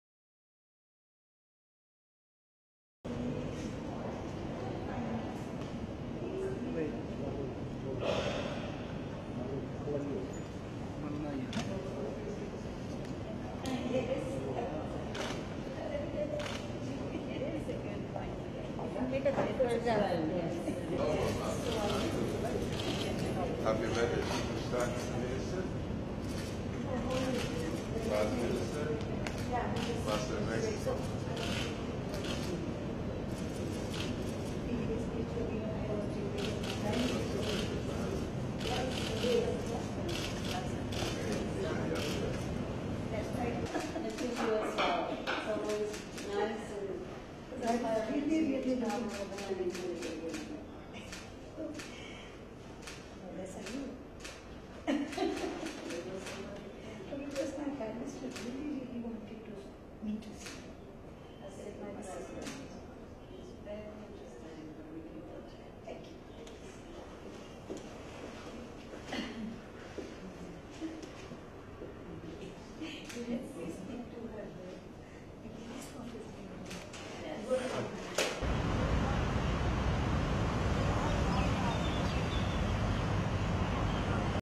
12 Mar, 2016 - Mexican Foreign Minister Claudia Ruiz Massieu meets Indian counterpart Sushma Swaraj
12 Mar, 2016 (ANI) - Mexican Foreign Minister, Claudia Ruiz Massieu, discusses bilateral relations in a meeting with meets her Indian counterpart, Sushma Swaraj, in New Delhi.